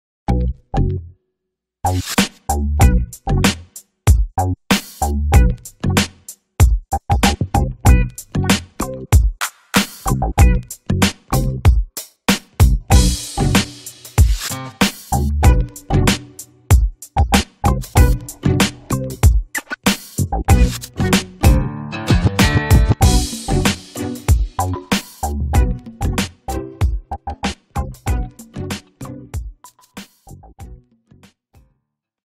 WHO’S RICHER? - Paris Hilton or Jay Z? - Net Worth Revealed!
Who’s worth more? How much money do Paris Hilton and Jay Z have? We reveal their net worth and see who’s the richest between the two!